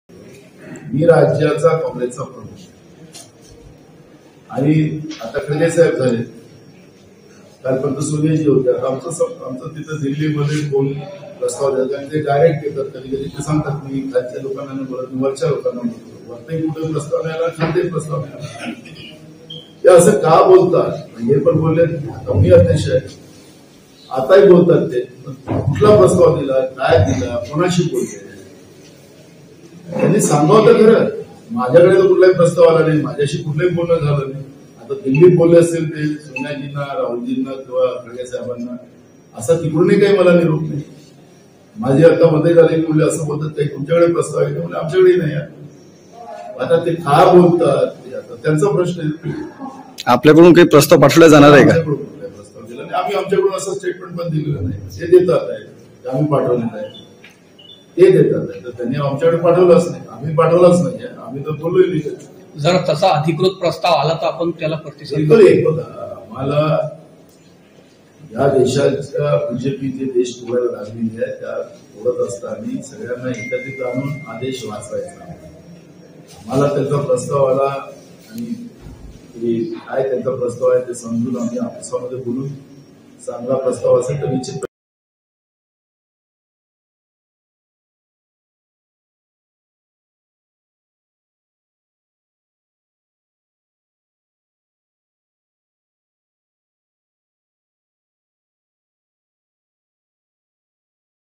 Nana Patole : वंचितकडून युतीच्या प्रस्तावावर नाना पटोलेंचे सूचक विधान |LetsUpp Marathi
युती संदर्भात वंचितकडून अद्यापतरी कुठलाही प्रस्ताव आलेला नाही. मात्र चांगला प्रस्ताव आल्यास त्यावर नक्कीच विचार करू, अशी सूचक प्रतिक्रिया काँग्रेसचे प्रदेशाध्यक्ष नाना पटोले यांनी दिली.